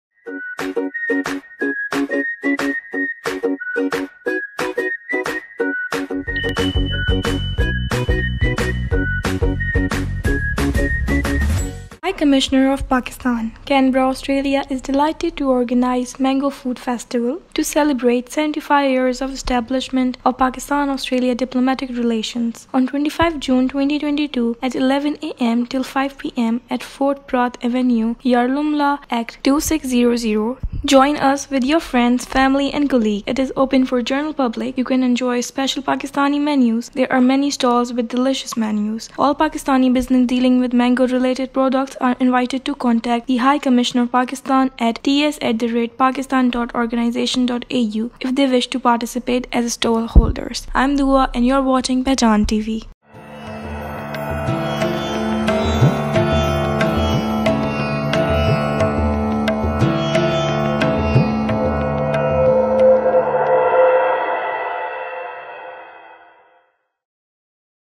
High Commissioner of Pakistan, Canberra Australia is delighted to organize Mango Festival.
To celebrate 75 years of establishment of Pakistan-Australia diplomatic relations.
 On 25 June 2022 at 11 AM till 5 PM at 4 Perth Avenue, Yarralumla, Act 2600. Join us with your friends, family, and colleagues.
 It is open to the general public. You can enjoy a special Pakistani menu. There are many stalls with delicious menus.
All Pakistani Businesses dealing with mango-related products are invited to contact the High Commission of Pakistan at If they wish to participate as stall holders.